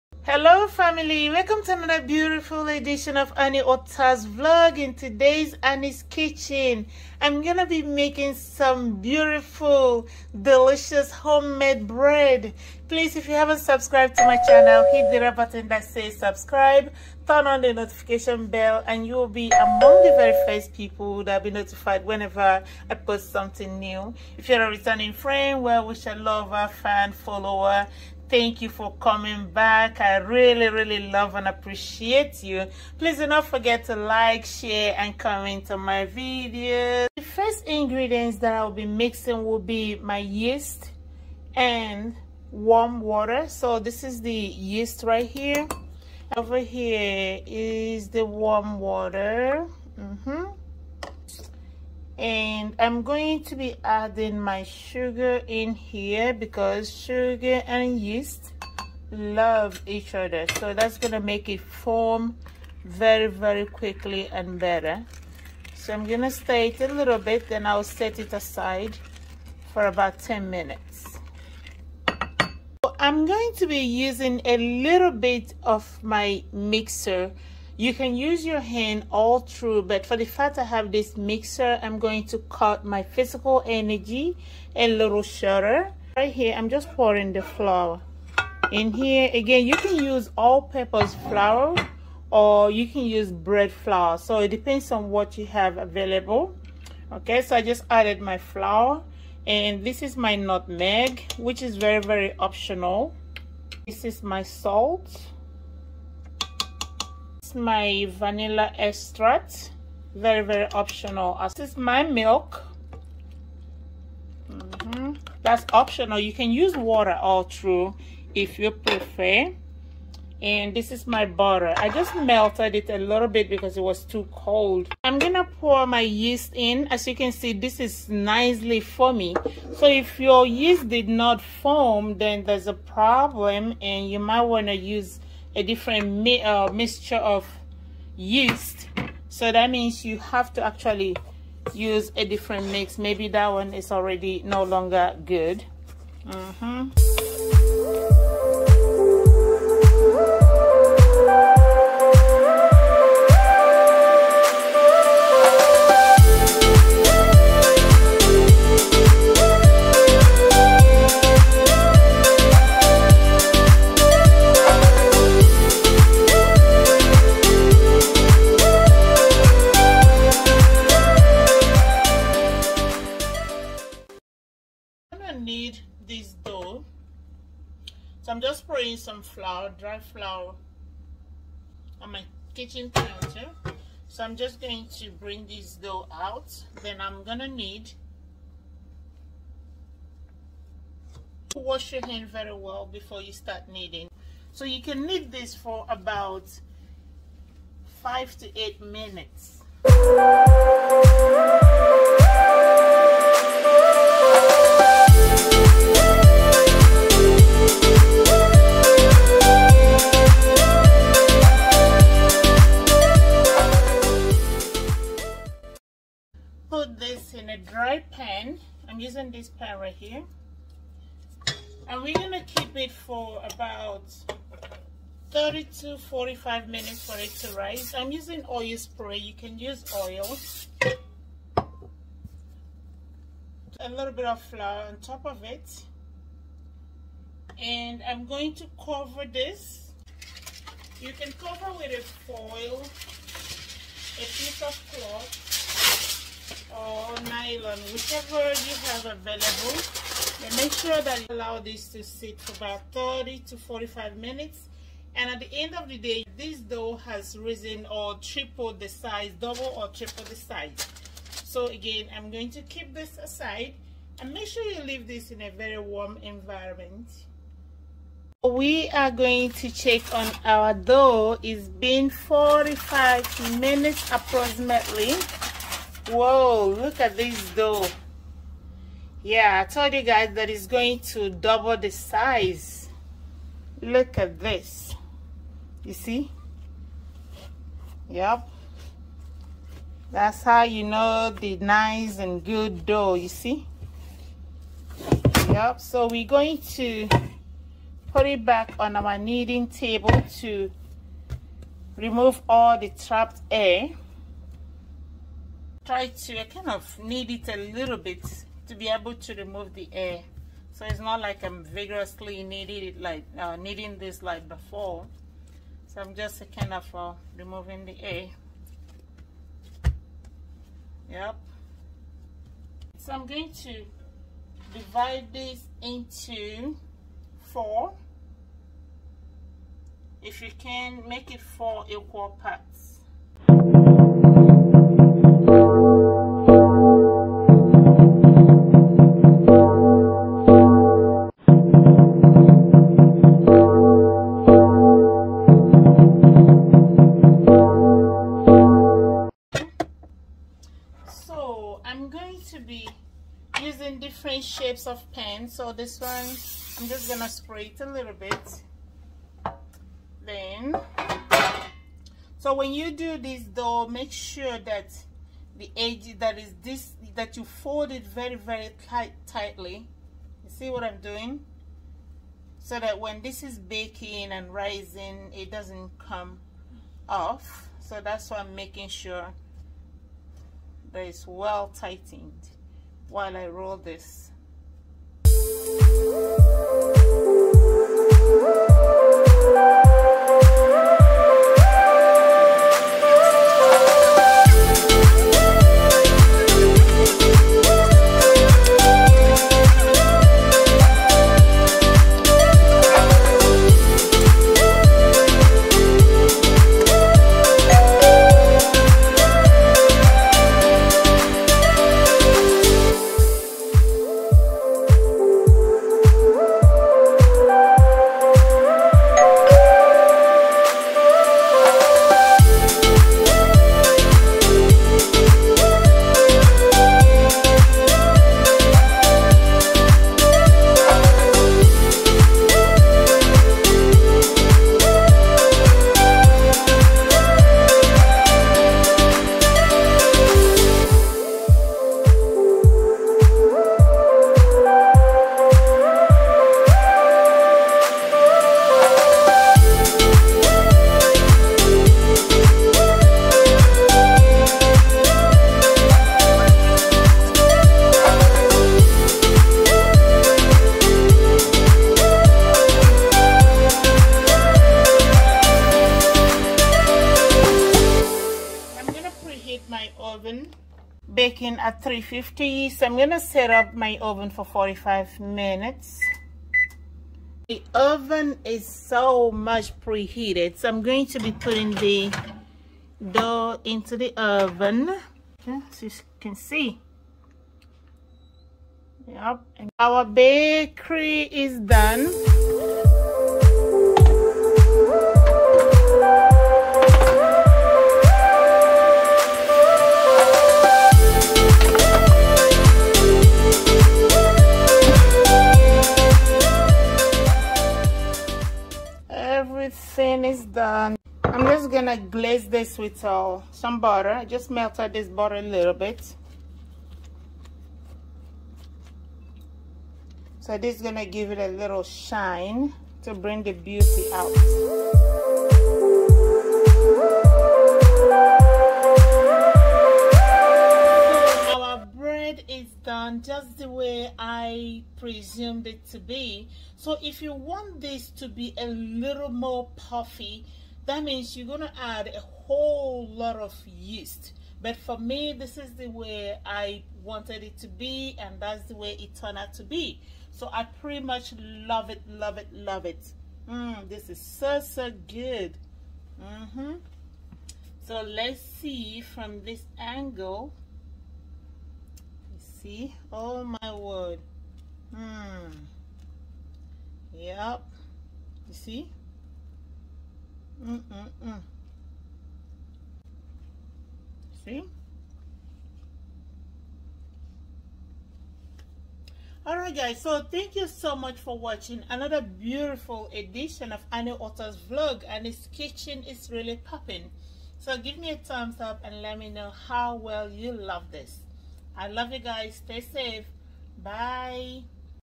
HOW TO MAKE A DELICIOUS FRENCH BREAD 🥖 🥐
4 Cups of Bread flour or All Purpose Flour
2 TBSP of Active Dry Yeast
1 Cup of warm water
1 TBSP of Sugar
4 TBSP of Unsalted Butter
1/4 Tsp of Salt
1/2 of Nutmeg
1 Cup of Carnation Evaporated Milk
1 TBSP Vanilla Extract
Cooking oil, spray ir butter greasing or glazing
Bake @ 350° for 30-45mins or until desired golden-brown